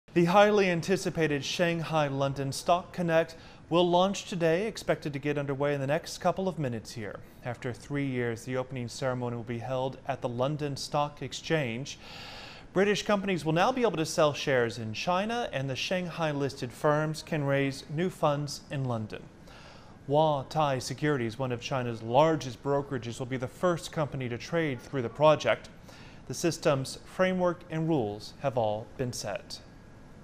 Shanghai London Stock Connect launched in London Stock Exchange
After more than three years in the making,  the highly-anticipated Shanghai-London Stock Connect has launched in London. The launch ceremony was held at the London Stock Exchange.